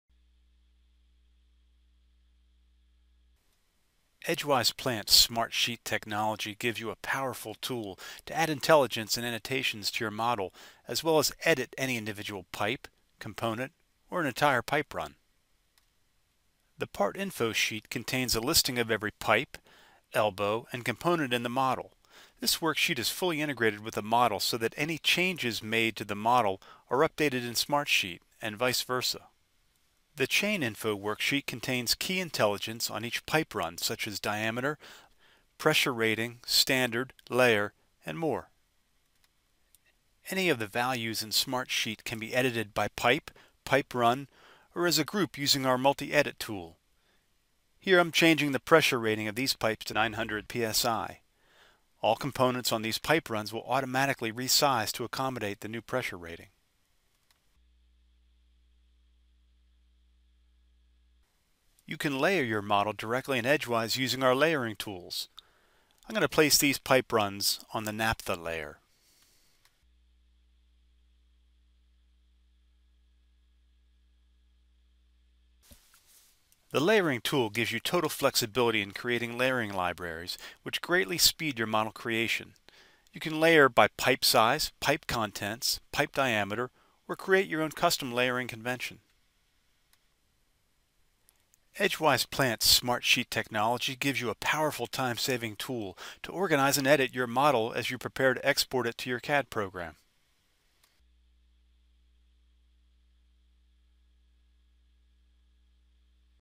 SmartSheet Technology Captures Model Intelligence
EdgeWise Plant's SmartSheet technology gives you powerful tools to add intelligence and annotations to your model as well as edit any individual pipe, component or an entire pipe run